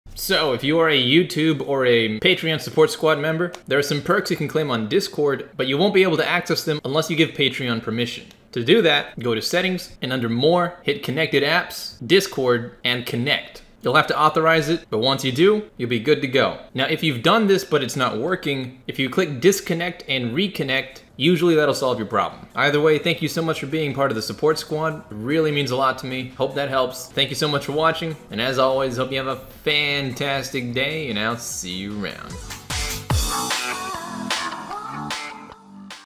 Connect Discord To Patreon (In 30 SECONDS!!)
Here's how you connect your Patreon Perks to Discord in 30 seconds-!!